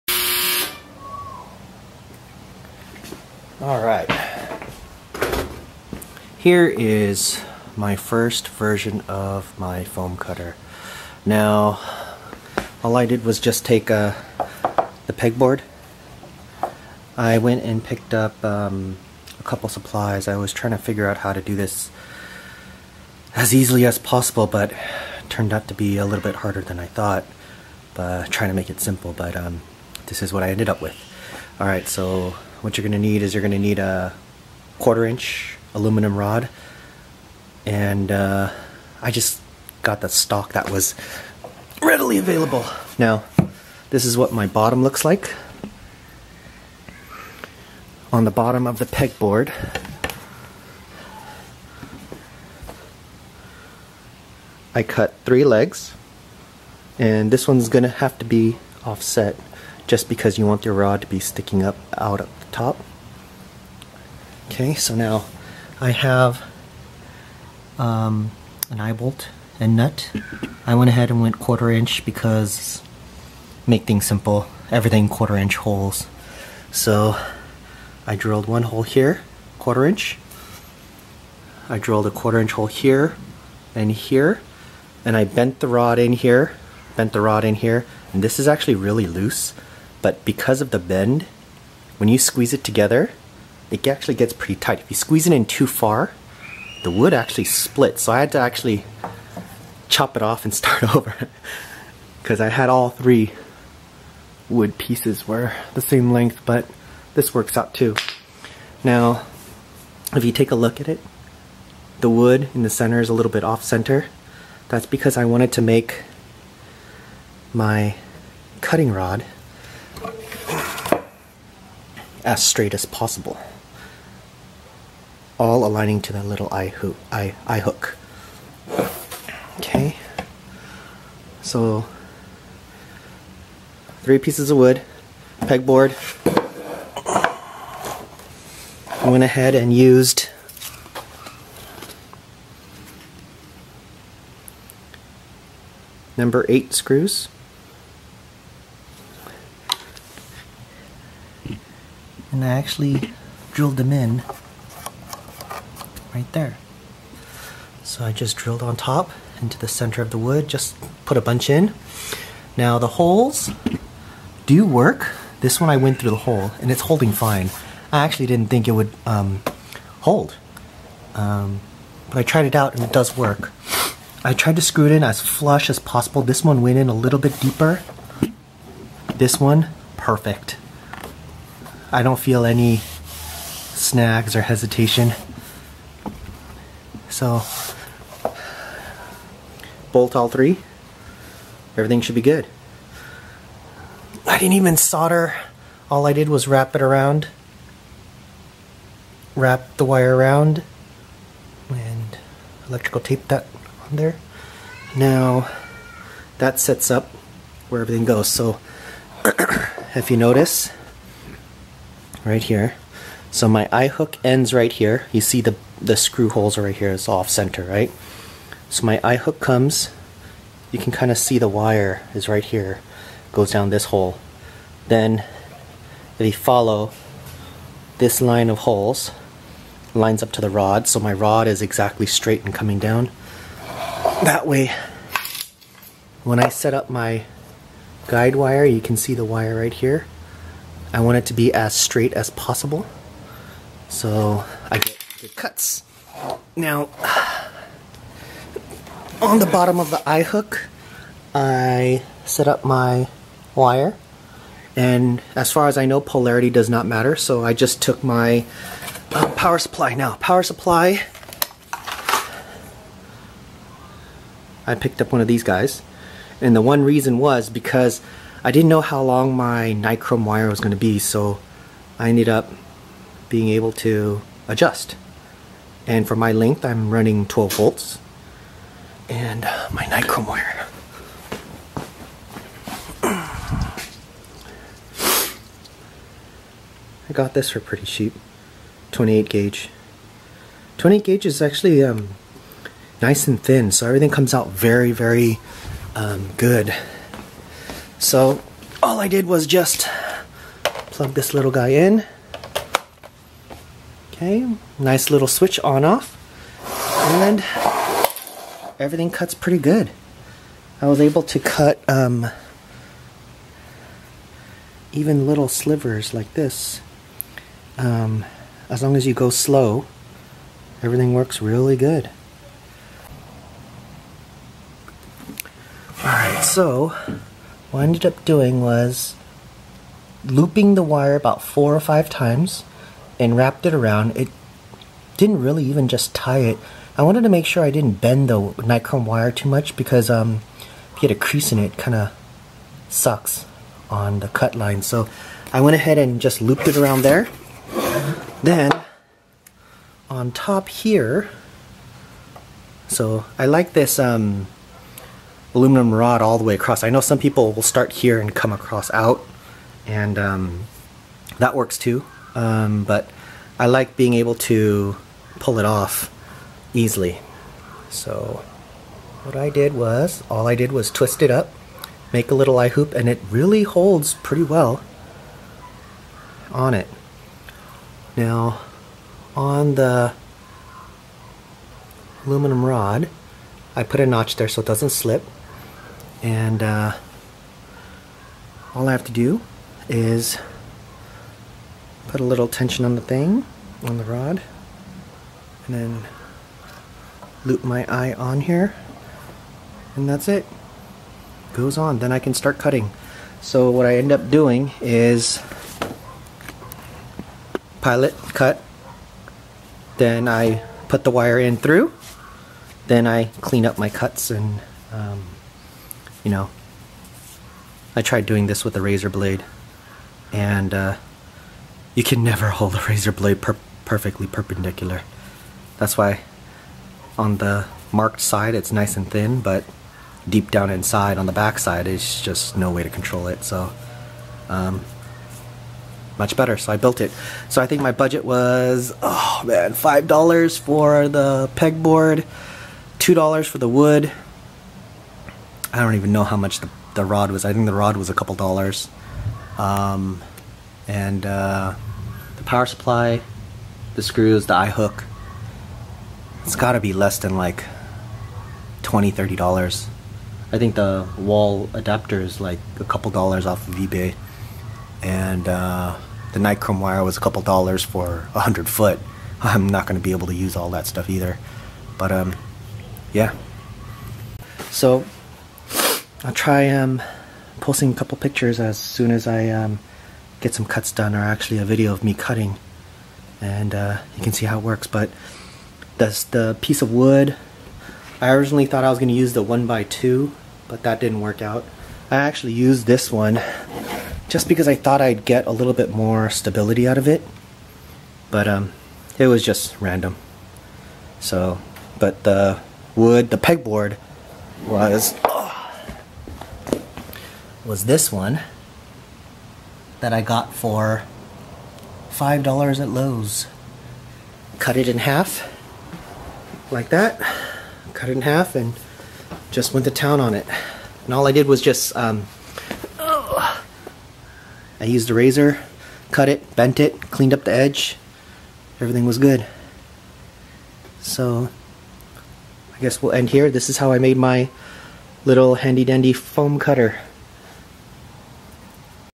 Custom Pelican Case Project: Part 1 - Foam Cutter DYI Build Setup
i had this idea to make a case that holds all of my handgun stuff for when i go to the range, take classes, and kinda show off a little when i see some friends.

part 1 is to make a foam cutter.

basic supplies:
peg board
1" wood screws
1/4 eye loop and bolt with washers
1/4 aluminum rod
1"x1"x8' foot wood beam
power supply DC
nichrome wire

i review how i built my simple foam cutter, i wanted to make a nice case.  i was told i can use a knife and cut in to frozen foam.  the big problem i had was that i couldn't even get a straight cut or perpendicular cut.  so yea, i saw this built by other people and i put my take on it.  very simple.  if i could do it again, it would take probably like 1 hour to build.  very stable.